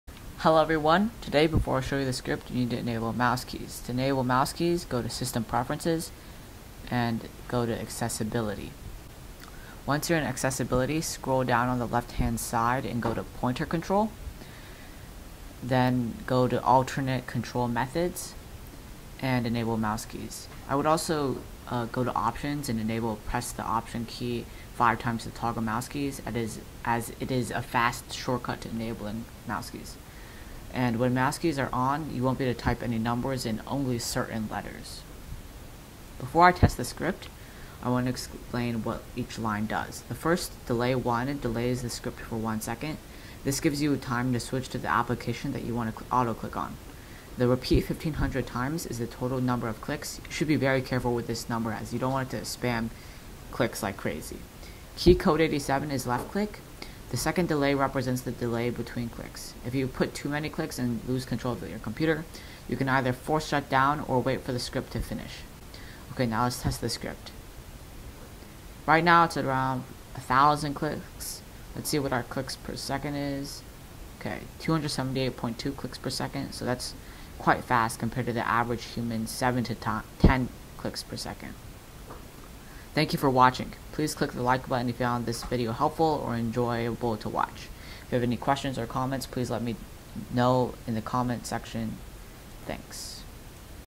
FREE Auto Clicker for Mac | 278.2 CPS
A simple and easy autoclicker for Mac, using Apple Script Editor (which is pre-installed on every Mac). If you have any questions/comments/suggestions please fee free to comment or join or Discord (below)!

Here's the script: (you can copy and paste and adjust the numbers to your liking):

tell application "System Events"

delay 1

key code 87

delay 0

end repeat
end tell